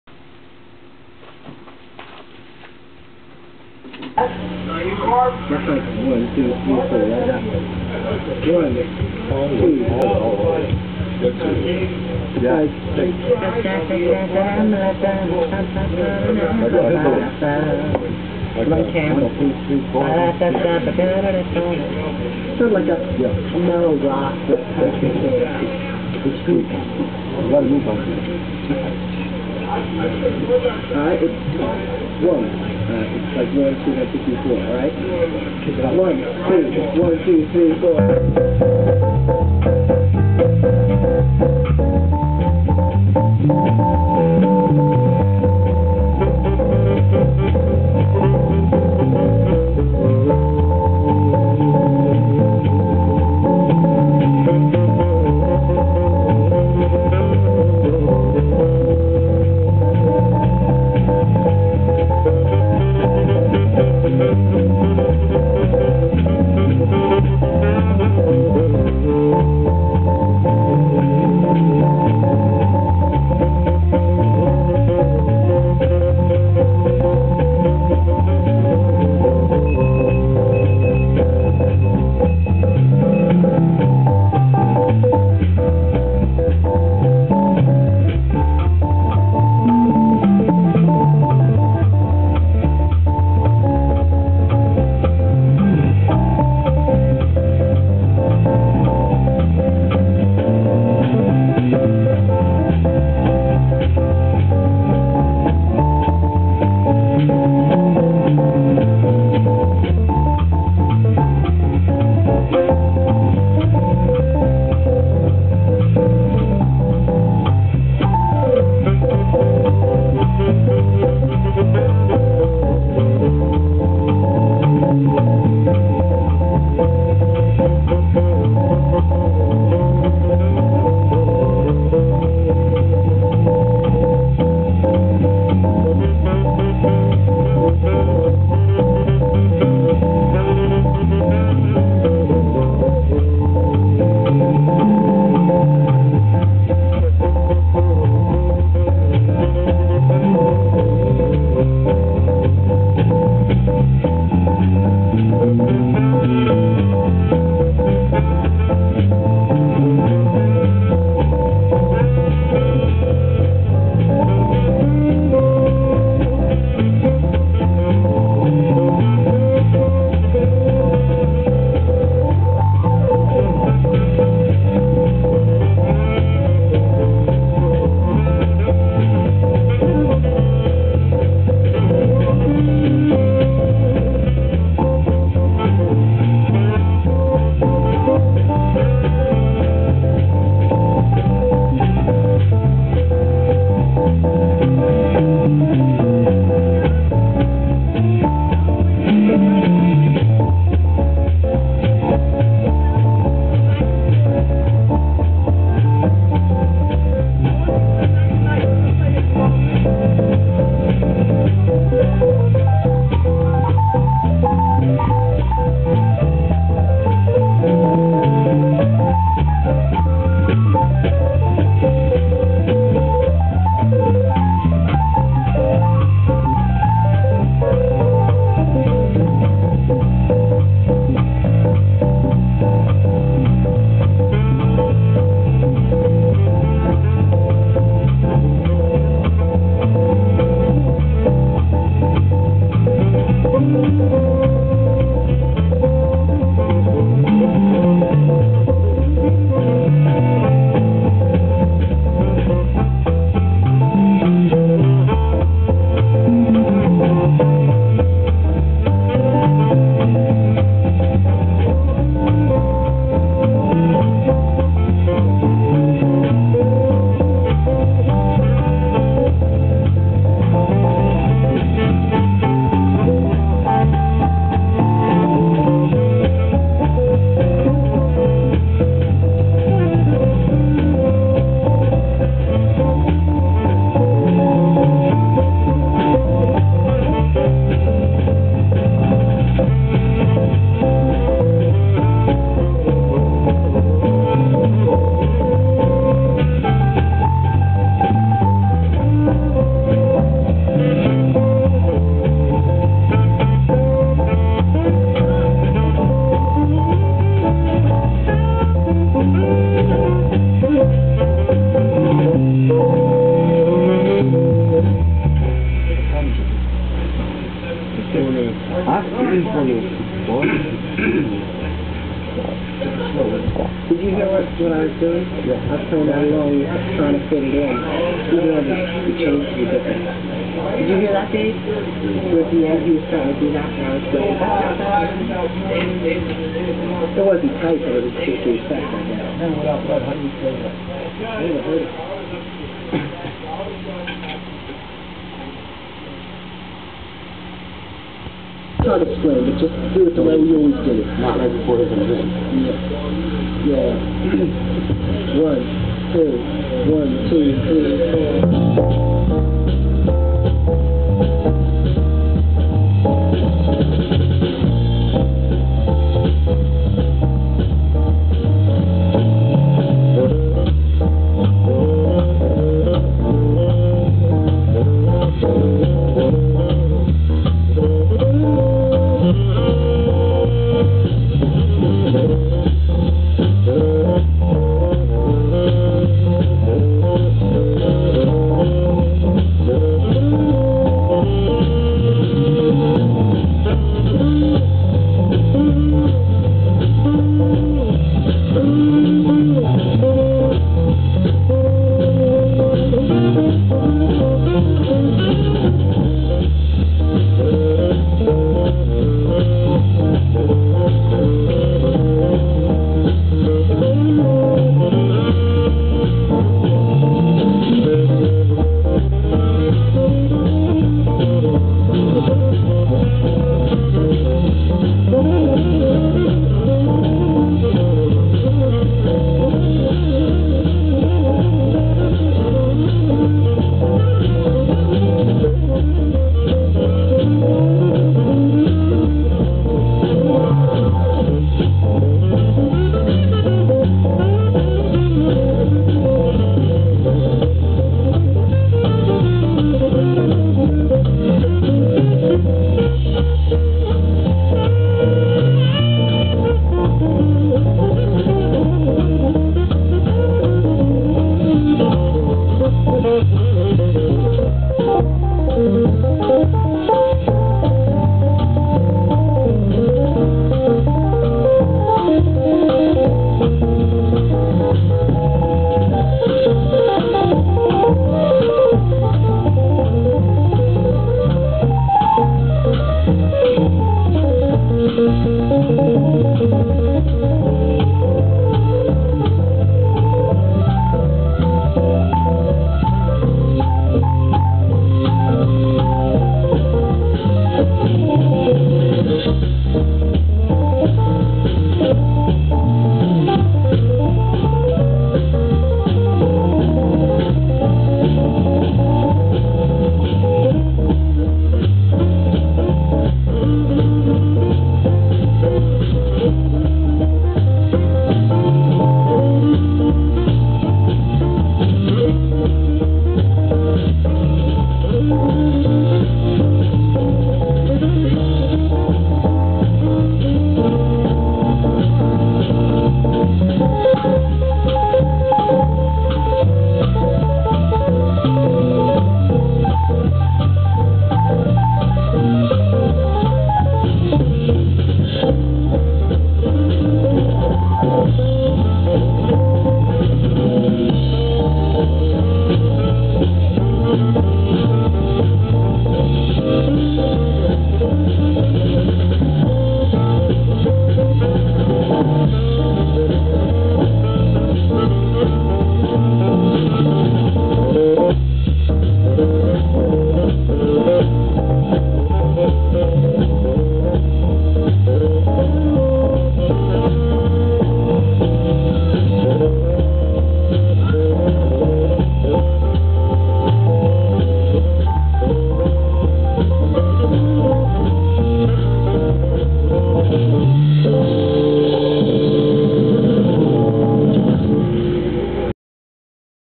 Andy Schmidt 2008 (Grammy winner Dave Kikoski fast at 12 yrs old)
From 1973. "2011 Grammy-winner" Dave K.(on piano at 12 yrs old) and his Dad(sax) w/ my brother Dave(drums) + me(bass)